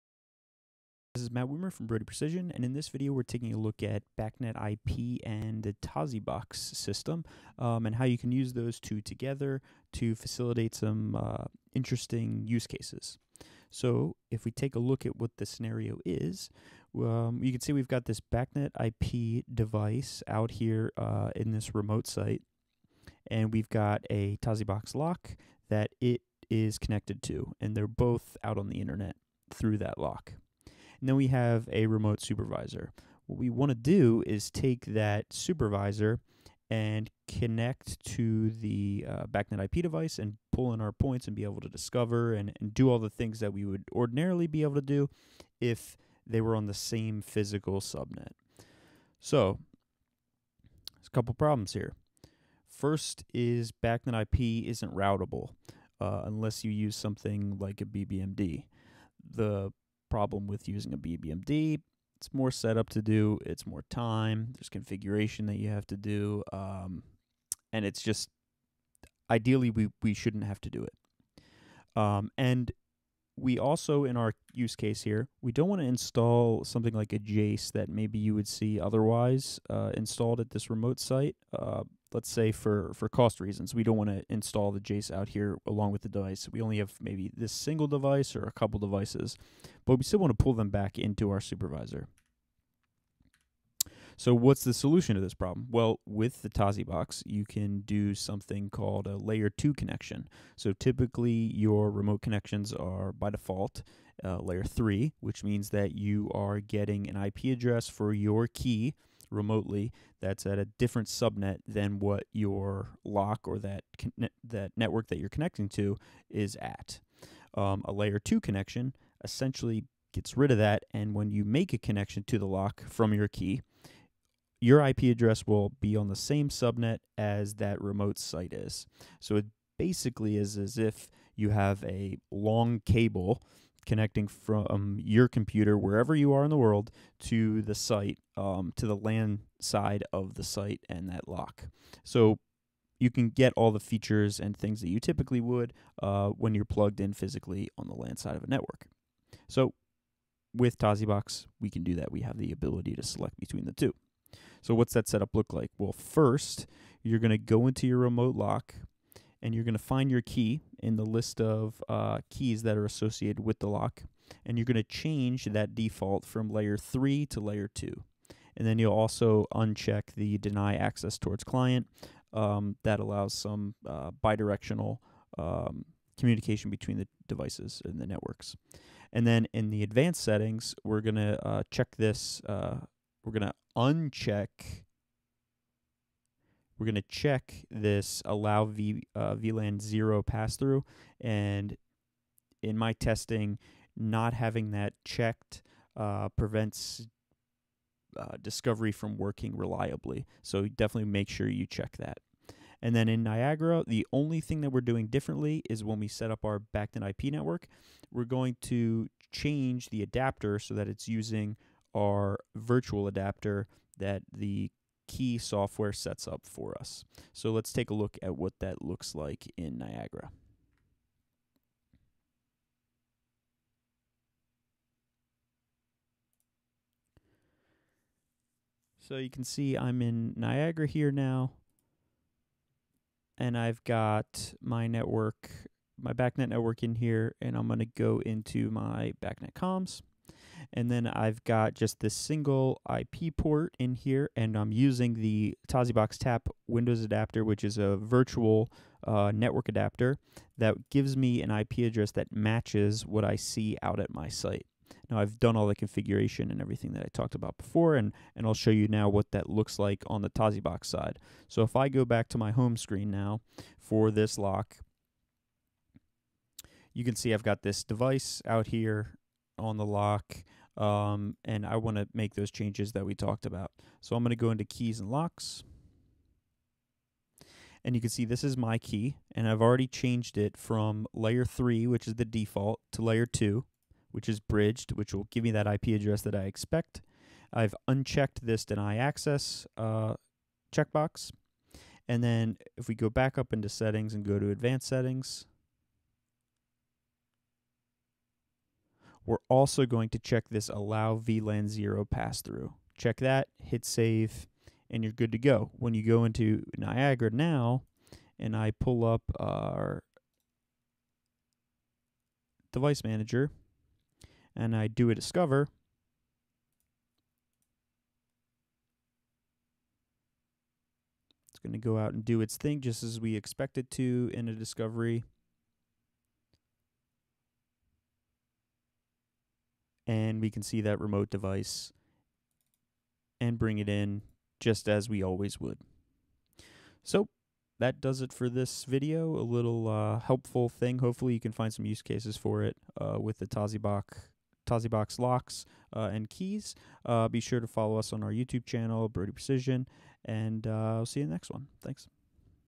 Tosibox & Bacnet IP
In this video, we take a look at using Tosibox with Bacnet IP and why that might be advantageous for certain projects.